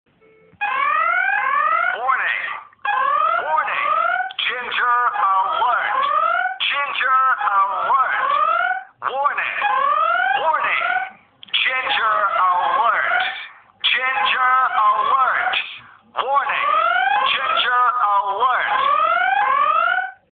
ginger alarm
a funny vid with me friend ginge. Filmed in bedlington and near Ashington ,blyth, newcastle